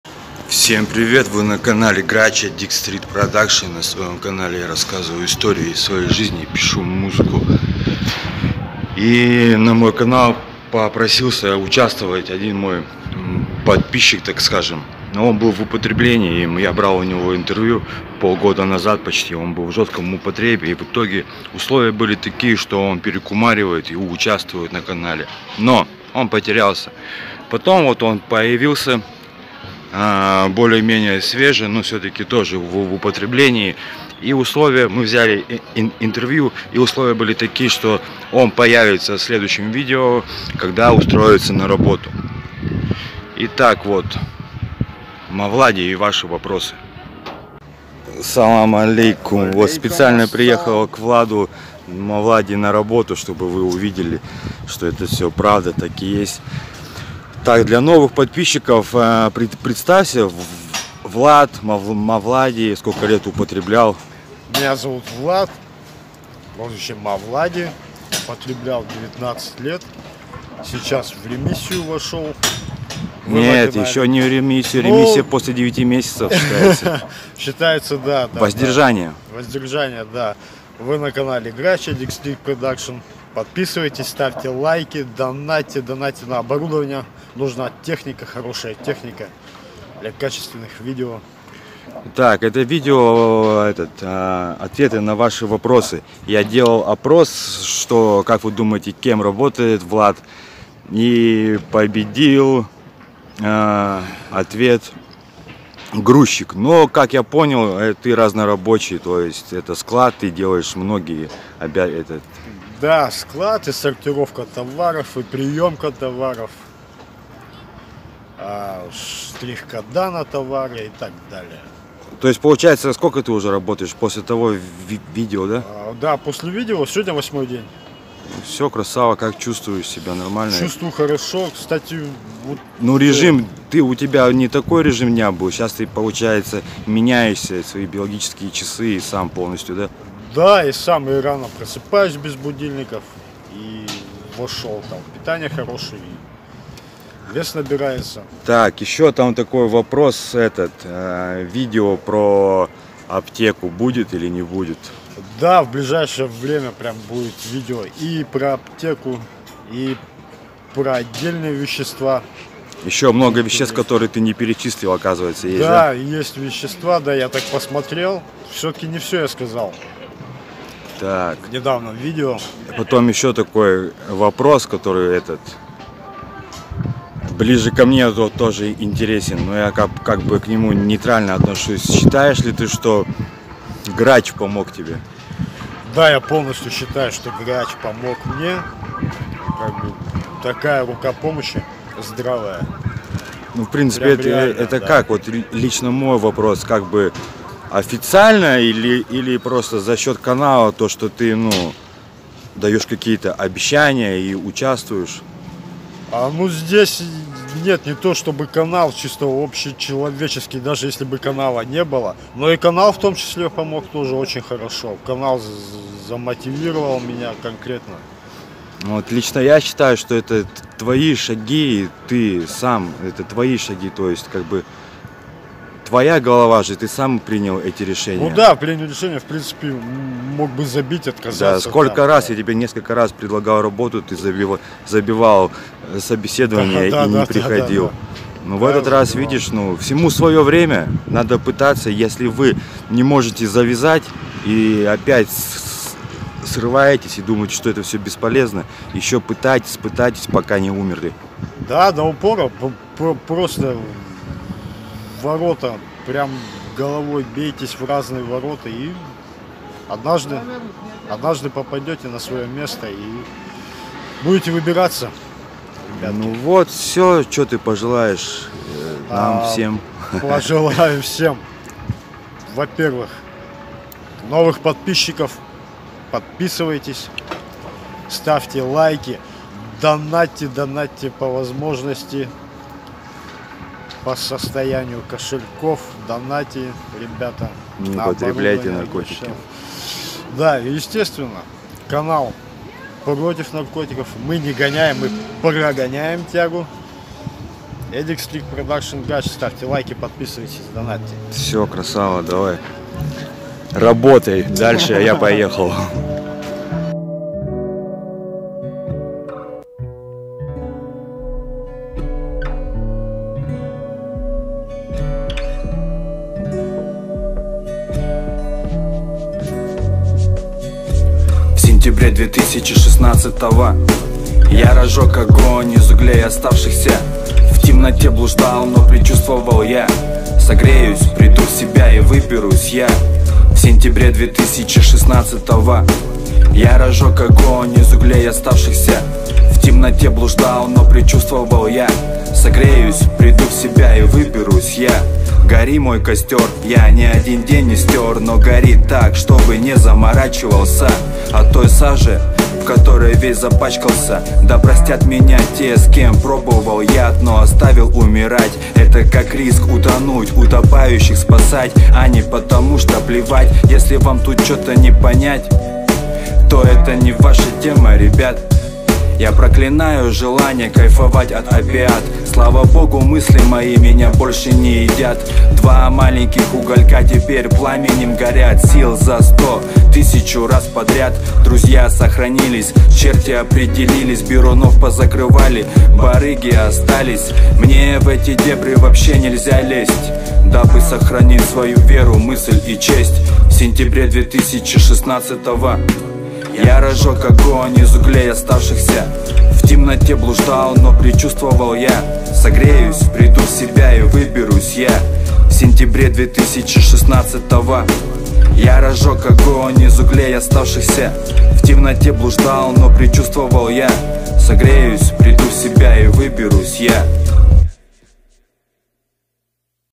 БРОСИЛ НАРКОТИКИ | ГРАЧ | МОВЛАДИ
ЭТО ВИДЕО О ЗАВИСИМОМ ЧЕЛОВЕКЕ.
СОХРАНИ ТРЕЗВОСТЬ
ФИНАНСОВАЯ ПОДДЕРЖКА КАНАЛА:
Киви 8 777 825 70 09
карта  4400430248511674